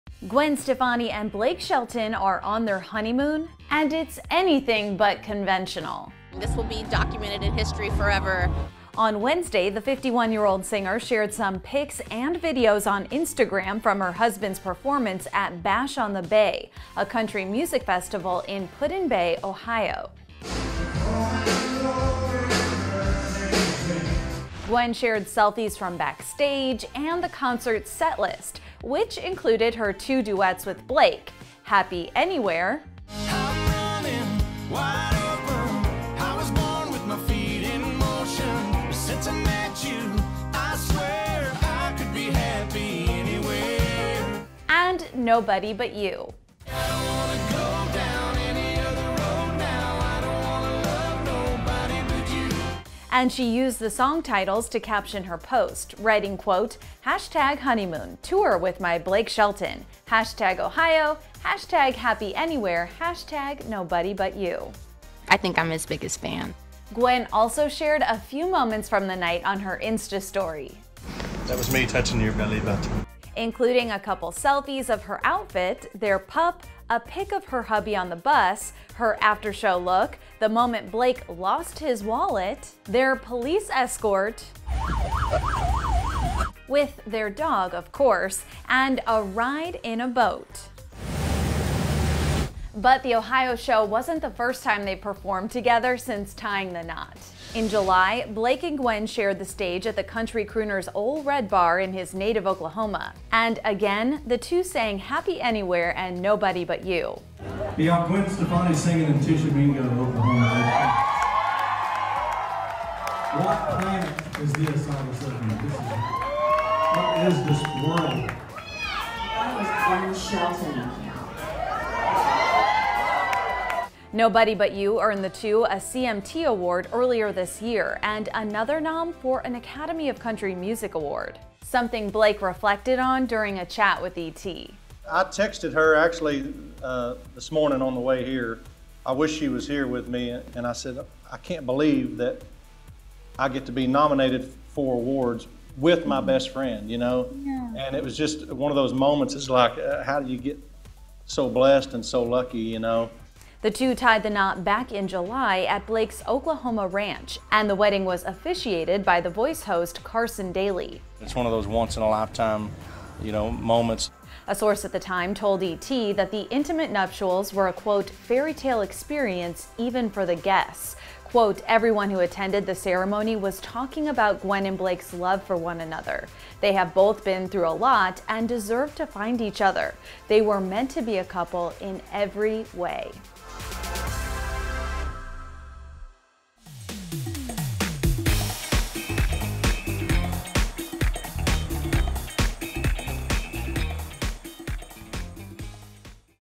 Go Inside Gwen Stefani's HONEYMOON With Blake Shelton
Gwen Stefani and Blake Shelton just kicked off their honeymoon by performing at a country music festival in Ohio.